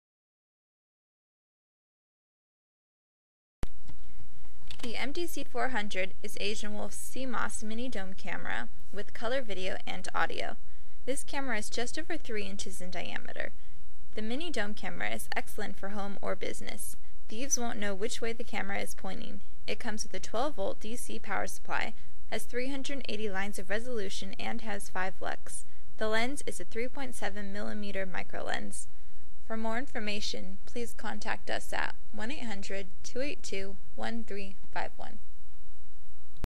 Asianwolf cmos 380 tv lines mini dome camera w/color video from asianwolf.com
The mdc-400 is asianwolf's high quality mini dome camera with color video and audio.  This camera is just over 3 inches in diameter.  This mini dome camera is excellent for home or business. Thieves won't know which way the camera is pointing! It comes with a 12V DC power supply, has 380 lines of resolution and has 5 LUX.  The lens is a 3.7mm microlens.